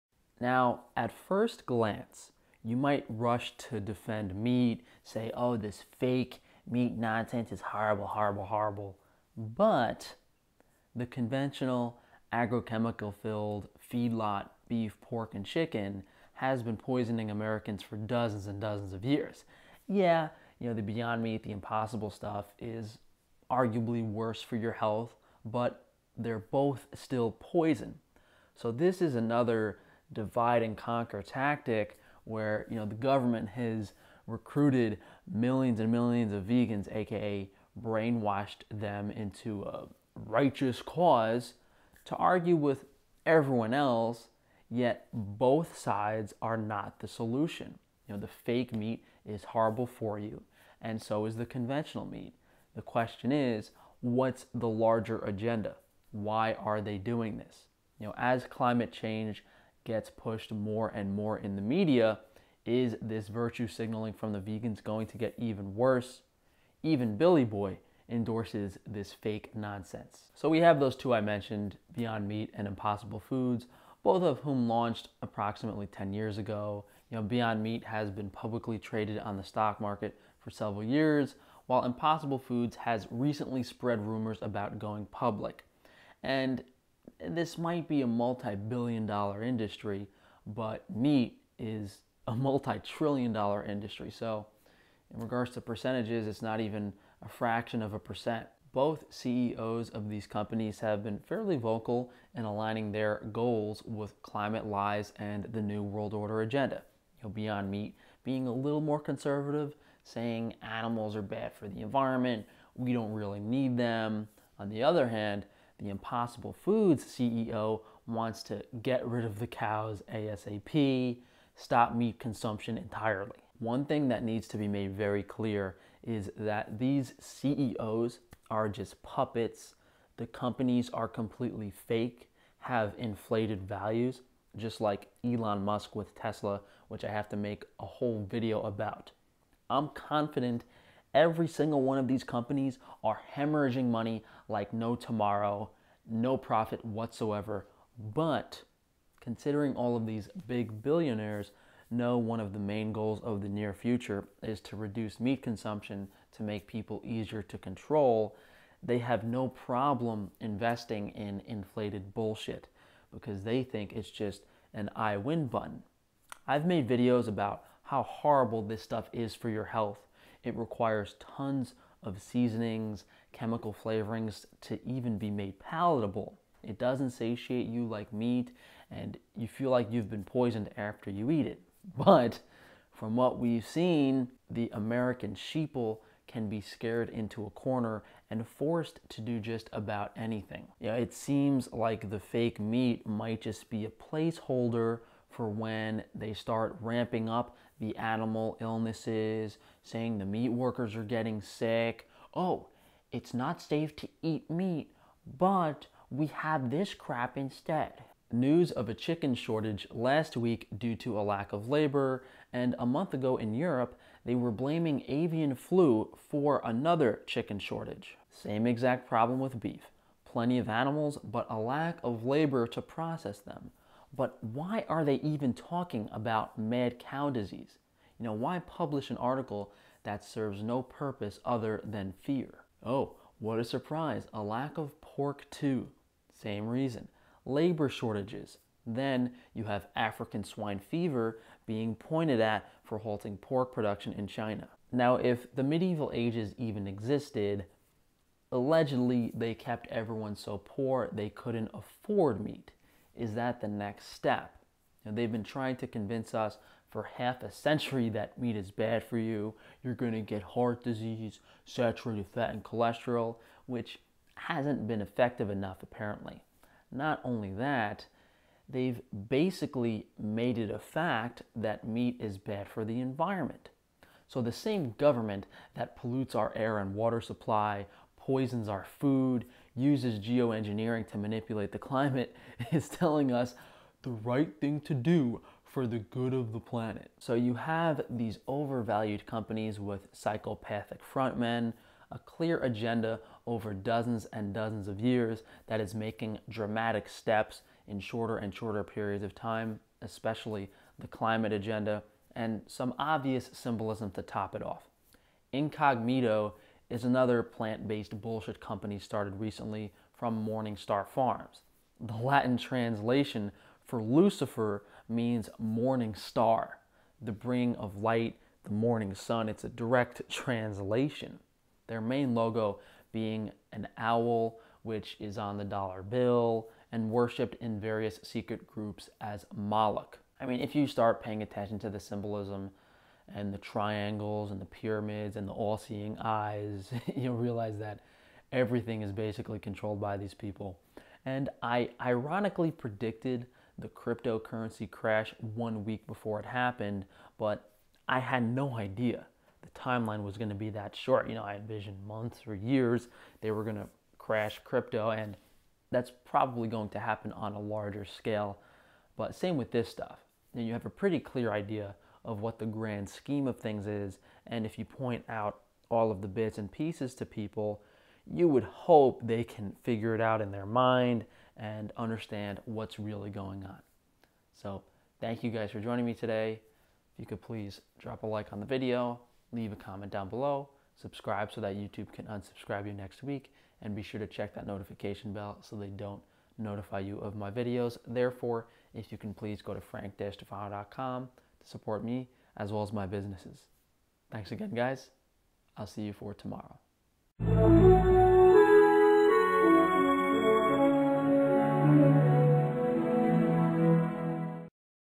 Is FAKE MEAT here to stay? What's the REAL AGENDA!?
00:00 - Carnivore VS Vegan?
01:10 - Beyond Meat and Impossible Foods
02:03 - All scam companies
03:10 - Is it a smaller component of the Agenda?
04:47 - Symbolism is the icing on the cake
06:15 - Thanks for joining